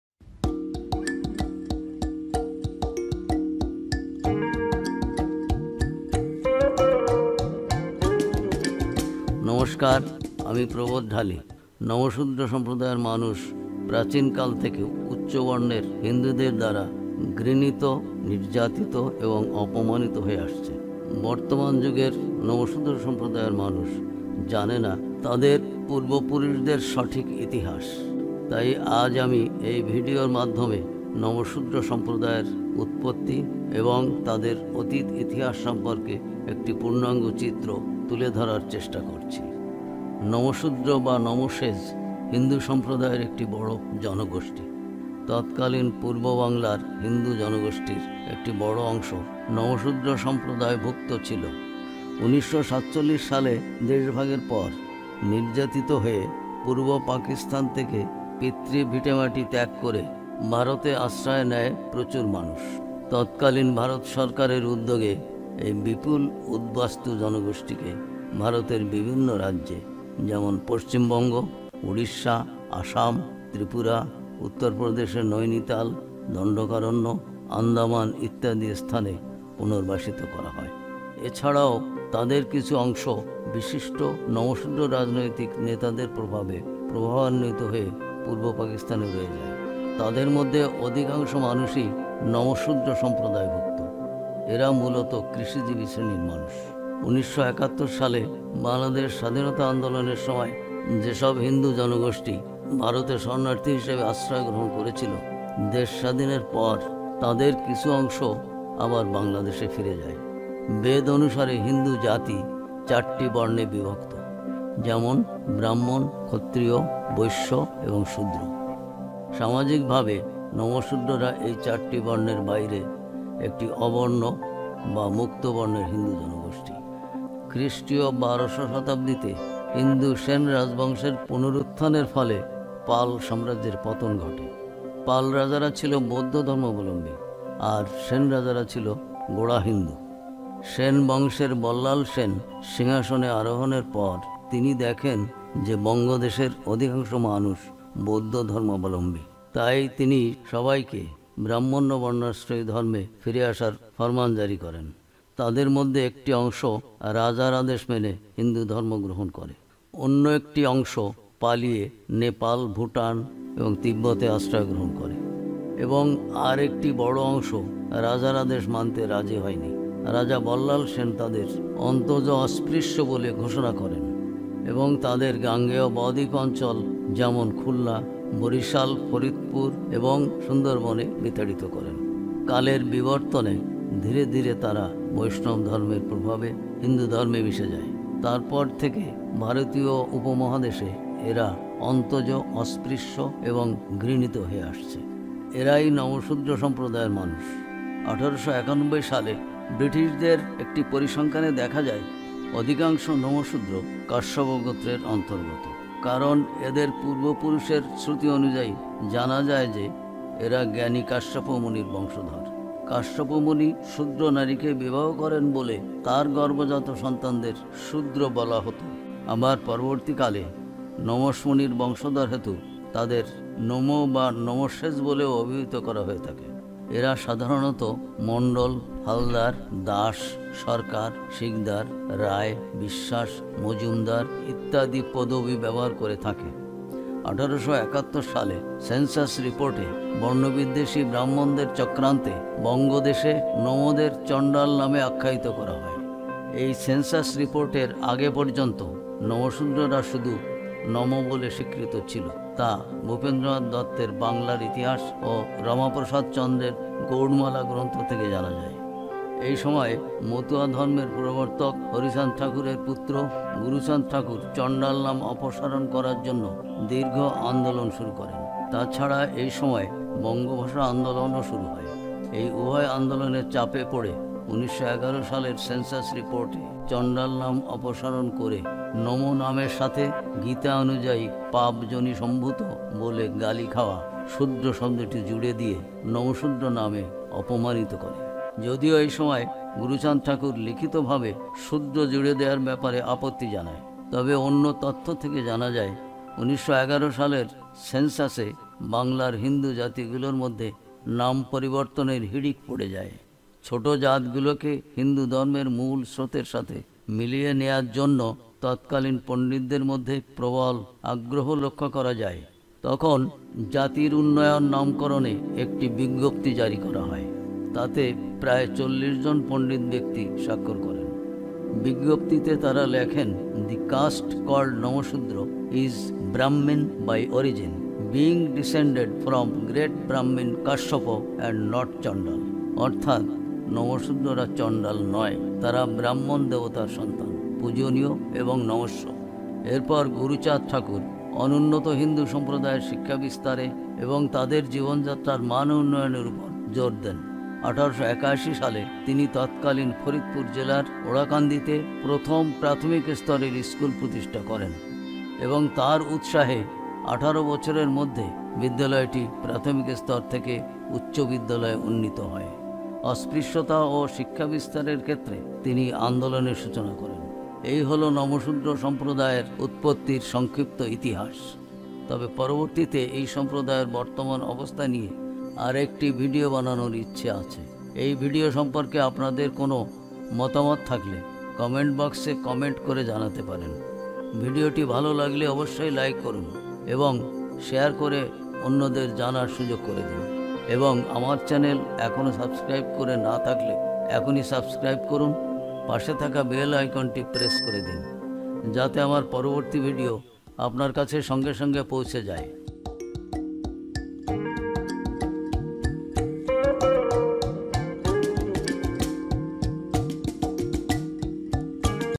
নমঃশূদ্র সম্প্রদায়ের প্রাচীন ইতিহাস | Ancient history of Namahsudra community
নমঃশূদ্র সম্প্রদায়ের মানুষ প্রাচীন কাল থেকে উচ্চ বর্ণের হিন্দুদের দ্বারা ঘৃণিত, নির্যাতিত এবং অপমানিত হয়ে আসছে। বর্তমান যুগের নমঃশূদ্র সম্প্রদায়ের মানুষ জানে না তাদের পূর্বপুরুষদের সঠিক ইতিহাস। তাই আজ আমি এই ভিডিওর মাধ্যমে নমঃশূদ্র সম্প্রদায়ের উ tutৎপত্তি এবং তাদের অতীত ইতিহাস সম্পর্কে একটি পূর্ণাঙ্গ চিত্র তুলে ধরার চেষ্টা করছি।

The people of the Namahsudra community have been hated, persecuted and humiliated by the upper caste Hindus since ancient times. The people of the Namahshudra community of the present age do not know the exact history of their ancestors. So today I am trying to give a complete picture about the origin of Namasudra community and their past history through this video.